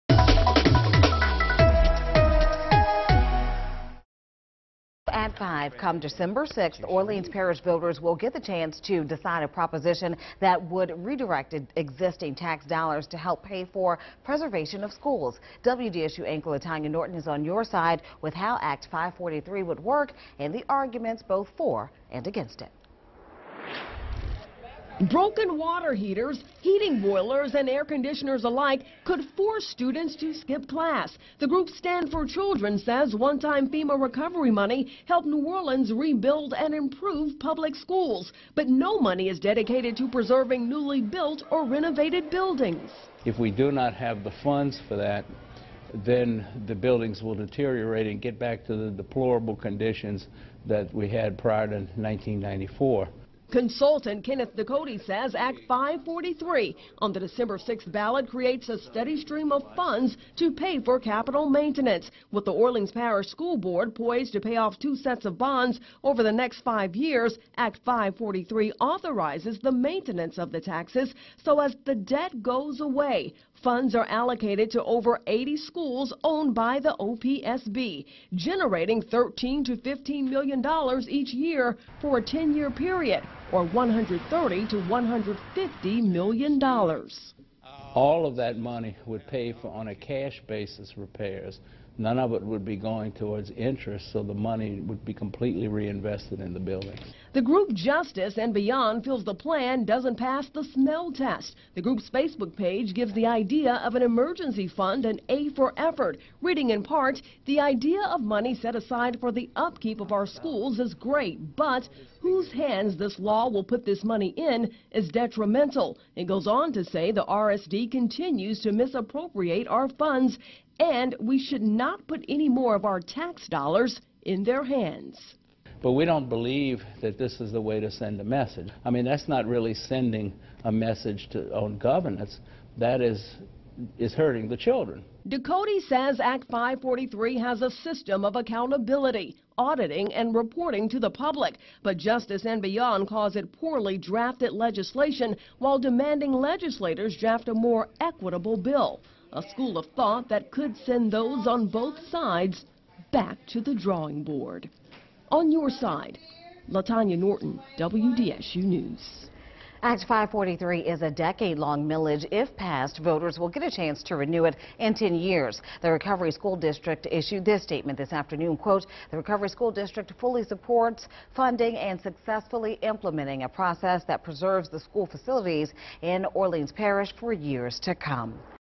Groups voice opinions on proposed act aimed at funding Orleans Parish schools
Broken water heaters, bathroom fixtures, boilers and air conditioners are typically not the first things you think about when it comes to school.